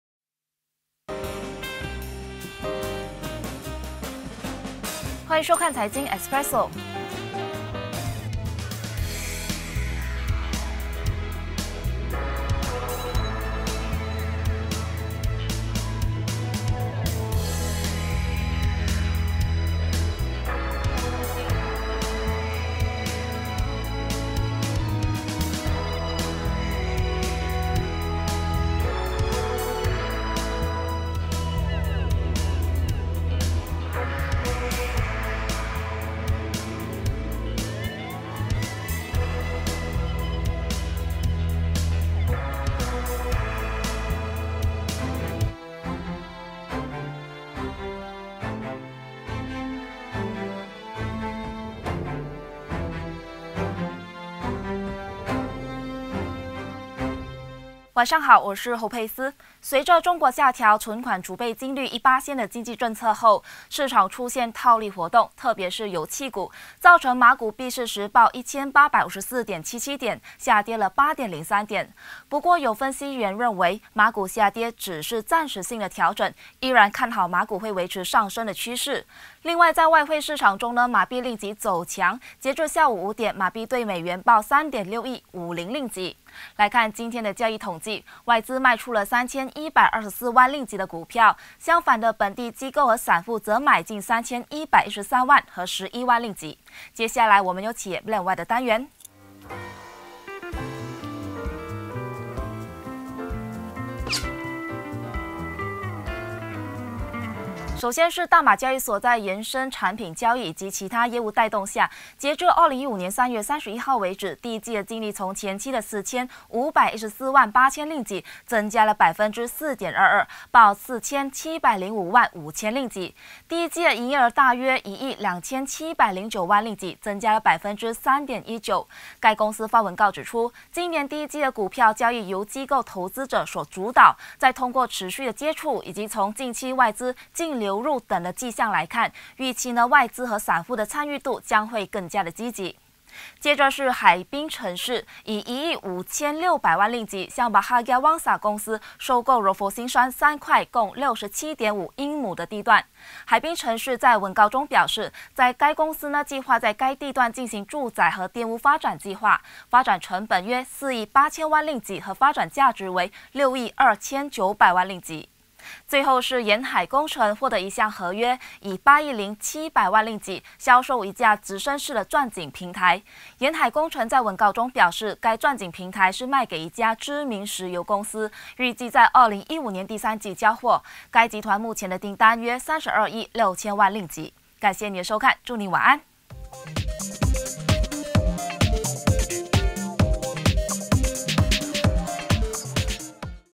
衍生品爭氣、Bursa多賺 20150422
大馬交易所發文告表示，從近期外資淨流入等跡象來看，預期外資和散戶的參與度將會更加積極。